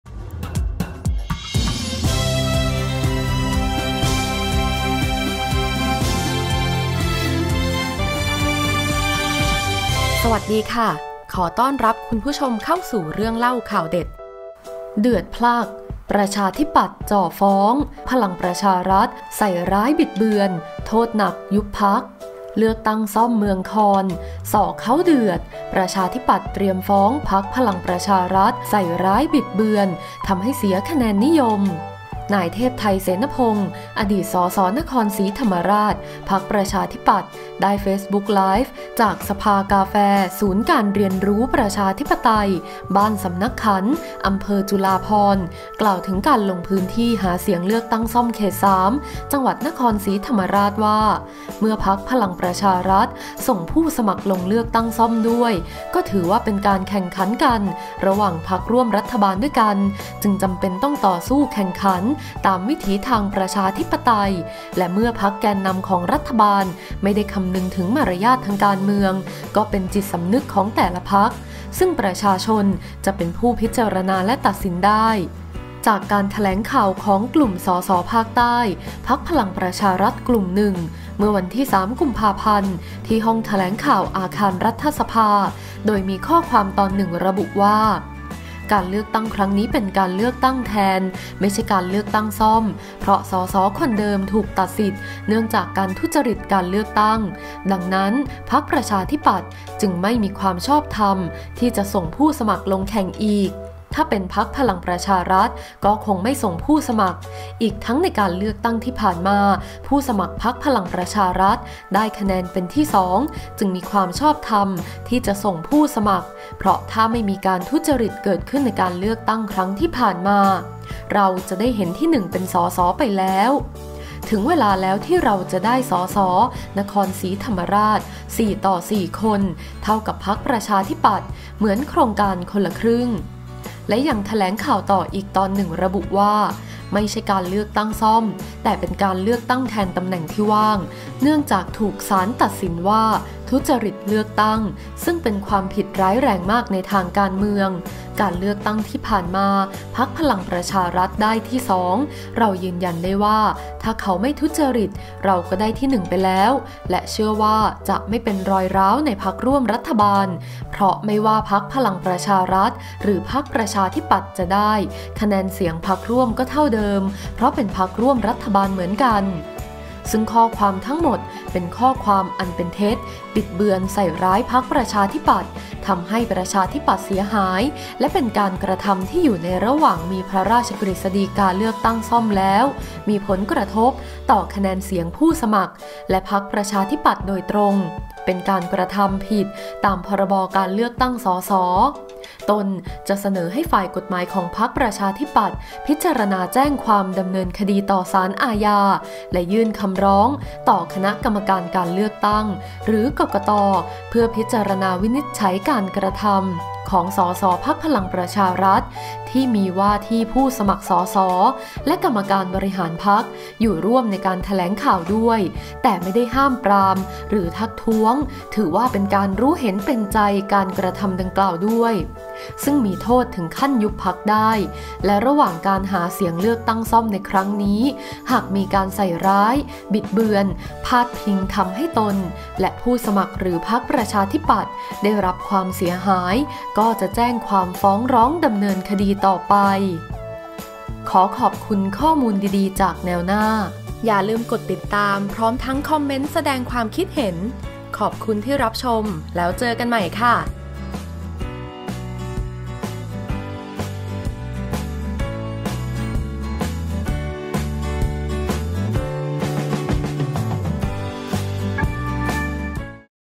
เดือดพลั่ก! "ประชาธิปัตย์" จ่อฟ้อง "พลังประชารัฐ" ใส่ร้ายบิดเบือน โทษหนัก "ยุบพรรค"
เลือกตั้งซ่อมเมืองคอน ส่อเค้าเดือด ประชาธิปัตย์ เตรียมฟ้อง พลังประชารัฐ ใส่ร้าย บิดเบือน ทำให้เสียคะแนนนิยม

นายเทพไท เสนพงศ์ อดีต ส.ส.นครศรีธรรมราช พรรคประชาธิปัตย์ ได้เฟซบุ๊กไลฟ์ จากสภากาแฟศูนย์เรียนรู้ประชาธิปไตย บ้านสำนักขัน อำเภอจุฬาภรณ์ กล่าวถึง การลงพื้นที่หาเสียงเลือกตั้งซ่อมเขต3
จ.นครศรีธรรมราช ว่า
เมื่อพรรคพลังประชารัฐ ส่งผู้สมัครลงเลือกตั้งซ่อมด้วย ก็ถือว่าเป็นการแข่งขันกันระหว่างพรรคร่วมรัฐบาลด้วยกัน จึงจำเป็นต้องต่อสู้แข่งขันตามวิถีทางประชาธิปไตย และเมื่อพรรคแกนนำของรัฐบาล
ไม่ได้คำนึงถึงมารยาททางการเมือง ก็เป็นจิตสำนึกของแต่ละพรรค ซึ่งประชาชนจะเป็นผู้พิจารณาและตัดสินได้
จากการแถลงข่าวของกลุ่ม ส.ส.ภาคใต้ พรรคพลังประชารัฐกลุ่มหนึ่ง เมื่อวันที่ 3 กุมภาพันธ์ ที่ห้องแถลงข่าวอาคารรัฐสภา โดยมีข้อความตอนหนึ่งระบุว่า
“การเลือกตั้งครั้งนี้เป็นการเลือกตั้งแทน ไม่ใช่การเลือกตั้งซ่อม เพราะ ส.ส.คนเดิมถูกตัดสิทธิ์เนื่องจากทุจริตการเลือกตั้ง ดังนั้น พรรคประชาธิปัตย์จึงไม่มีความชอบธรรม ที่จะส่งผู้สมัครลงแข่งอีก
ถ้าเป็นพรรคพลังประชารัฐก็คงไม่ส่งผู้สมัคร อีกทั้งในการเลือกตั้งที่ผ่านมา ผู้สมัครพรรคพลังประชารัฐ ได้คะแนนเป็นที่สอง จึงมีความชอบธรรมที่จะส่งผู้สมัคร เพราะถ้าไม่มีการทุจริตเกิดขึ้นในการเลือกตั้งครั้งที่ผ่านมา
เราจะได้ที่หนึ่งได้เป็น ส.ส.ไปแล้ว ถึงเวลาแล้วที่เราจะได้ ส.ส.นครศรีธรรมราช 4 ต่อ 4 คน เท่ากับพรรคประชาธิปัตย์ เหมือนโครงการคนละครึ่ง”
และยังแถลงข่าวต่ออีกตอนหนี่งระบุว่า "ไม่ใช่การเลือกตั้งซ่อม แต่เป็นการเลือกตั้งแทนตำแหน่งที่ว่าง เนื่องจากถูกศาลตัดสินว่า ทุจริตเลือกตั้ง ซึ่งเป็นความผิดร้ายแรงมากในทางการเมือง
การเลือกตั้งที่ผ่านมาพรรคพลังประชารัฐได้ที่สอง เรายืนยันได้ว่า ถ้าเขาไม่ทุจริตเราก็ได้ที่หนึ่งไปแล้ว และเชื่อว่าจะไม่เป็นรอยร้าวในพรรคร่วมรัฐบาล เพราะไม่ว่าพรรคพลังประชารัฐ
หรือพรรคประชาธิปัตย์จะได้ คะแนนเสียงพรรคร่วมก็เท่าเดิม เพราะเป็นพรรคร่วมรัฐบาลเหมือนกัน"
ซึ่งข้อความทั้งหมดเป็นข้อความอันเป็นเท็จ บิดเบือนใส่ร้ายพรรคประชาธิปัตย์ ทำให้ประชาธิปัตย์เสียหาย และเป็นการกระทำที่อยู่ในระหว่างมีพระราชกฤษฎีกาเลือกตั้งซ่อมแล้ว
มีผลกระทบต่อคะแนนเสียงผู้สมัครและพรรคประชาธิปัตย์โดยตรง เป็นการกระทำผิดตาม พรป.การเลือกตั้ง ส.ส.
ตนจะเสนอให้ฝ่ายกฎหมายของพรรคประชาธิปัตย์ พิจารณาแจ้งความดำเนินคดีต่อศาลอาญา และยื่นคำร้องต่อคณะกรรมการเลือกตั้ง(กกต.) เพื่อพิจารณาวินิจฉัยการกระทำของ ส.ส.พรรคพลังประชารัฐ
ที่มีว่าที่ผู้สมัคร ส.ส.และกรรมการบริหารพรรค อยู่ร่วมในการแถลงข่าวด้วย แต่ไม่ได้ห้ามปราม หรือทักท้วง  ถือว่าเป็นการรู้เห็นเป็นใจการกระทำดังกล่าวด้วย ซึ่งมีโทษถึงขั้นยุบพรรคได้
และระหว่างการหาเสียงเลือกตั้งซ่อมในครั้งนี้ หากมีการใส่ร้าย บิดเบือน พาดพิงทำให้ตน และผู้สมัคร หรือพรรคประชาธิปัตย์ ได้รับความเสียหาย ก็จะแจ้งความ ฟ้องร้องดำเนินคดีต่อไป

ขอขอบคุณข้อมูลดีๆจาก แนวหน้า